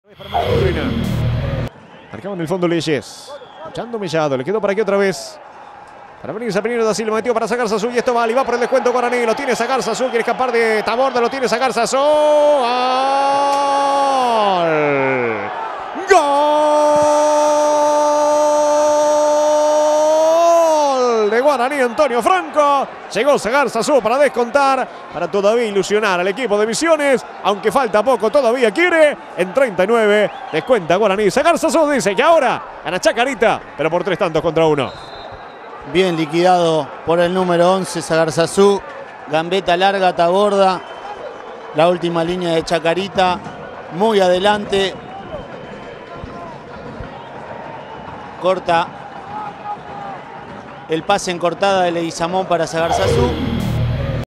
Gol de Sagarzazú. Chacarita 3 - Guaraní 1. Fecha 35. B Nacional 2015. FPT
El volante del equipo misionero picó entre los defensores, esquivó a Taborda y definió para el descuento.